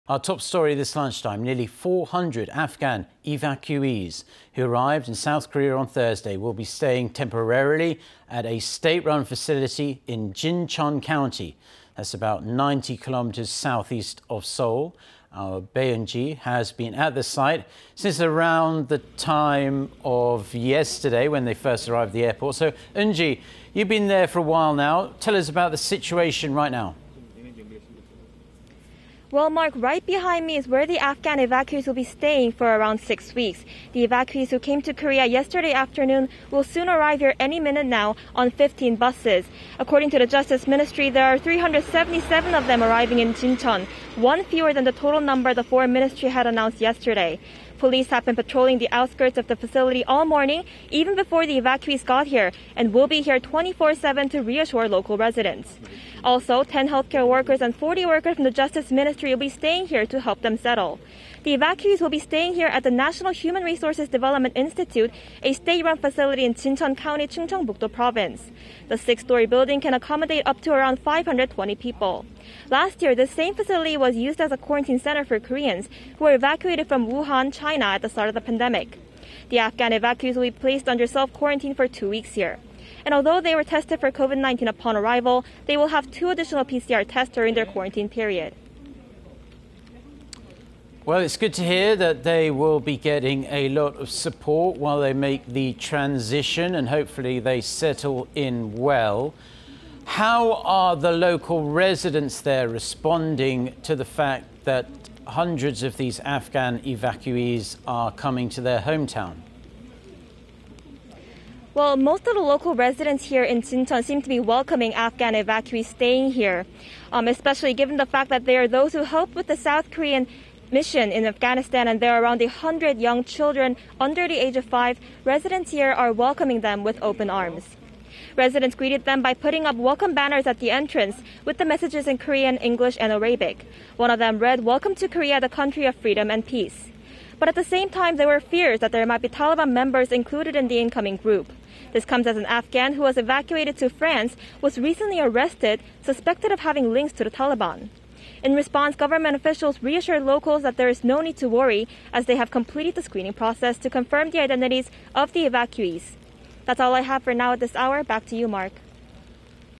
378 Afghan evacuees in S. Korea to be housed in Jincheon County starting Friday
오늘 오전 아프간인 378명 법무부 차관과 함께 진천으로... 이 시각 진천은?

Our top story this lunchtime.
Nearly 400 Afghan evacuees who arrived in South Korea on Thursday will be staying temporarily at a state-run facility in  Jincheon county,... about 90 kilometers southeast of Seoul.
Our Bae Eun-ji has been at the site since around the time they arrived.
So Eun-ji, you've been there for a while now... tell us about the situation.

Well Mark,... right behind me is where the Afghan evacuees will be staying,... for around 6 weeks.
The evacuees who came to Korea yesterday afternoon will soon arrive here on 15 buses.
According to the Justice ministry, there are 377 of them arriving in Jincheon,... one fewer than the total number the foreign ministry had announced yesterday.
Police have been patrolling the outskirts of the facility all morning even before the evacuees got here,... and will be here 24/7 to reassure local residents.
Also, 10 healthcare workers and 40 workers from the Justice Ministry will be staying here to help them settle.
The evacuees will be staying here at the National Human Resources Development Institute, a state-run facility in Jincheon County,  Chungcheongbukdo Province.
The 6-story building can accommodate up to around 520 people.
Last year, this same facility was used as a quarantine center for Koreans who were evacuated from Wuhan, China at the start of the pandemic.
The Afghan evacuees will be placed under self-quarantine for two weeks here.
And although they were tested for COVID-19 upon arrival,... they will have two additional PCR tests during their quarantine period.

It's good to hear that there will be a lot of support to help them settle.
How are local residents there responding to housing Afghan evacuees in their hometown?

Most of the local residents here seem to be welcoming the Afghans staying here in Jincheon.
Especially because they are those who helped with the South Korean mission in Afghanistan, and there are around a hundred young children under the age of 5,... these residents are ready to welcome them with open arms.
Residents greeted them by putting up welcome banners at the entrance, with the messages in Korean, English, and Arabic.
One of them read "Welcome to Korea, the country of Freedom and Peace."
But at the same time,... there were fears that there might be Taliban members included in the incoming group.
This comes as an Afghan, who was evacuated to France, was recently arrested,... suspected of having links to the Taliban.
In response, government officials reassured locals that there is no need to worry,... as they have completed the screening process to confirm the identities of the evacuees.
That's all I have for now at this hour.
Back to you, Mark.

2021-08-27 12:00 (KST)